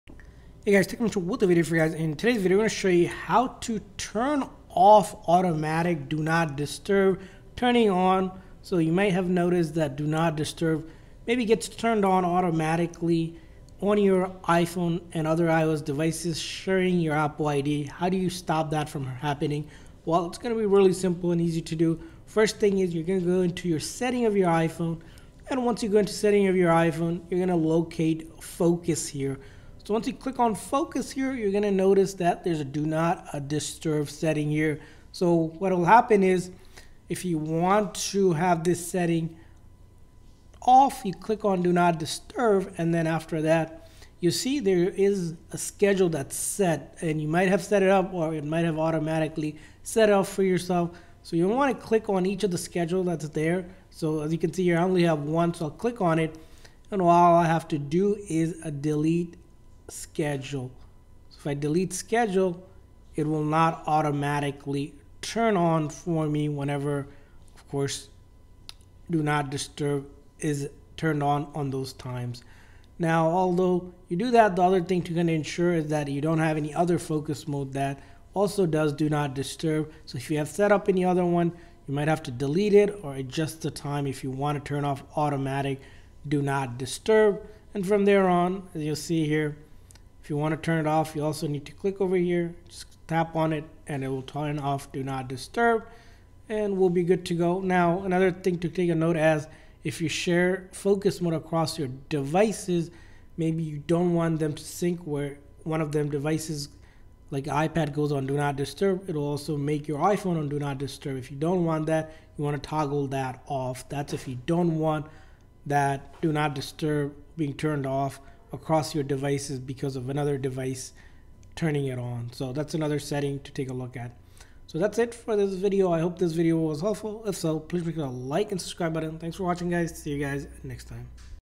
How to Turn Off Automatic Do Not Disturb on iPhone
Does your automatic Do Not Disturb turn on by itself? In this video we show you how to turn off automatic Do Not Disturb feature on your iPhone on the latest software update.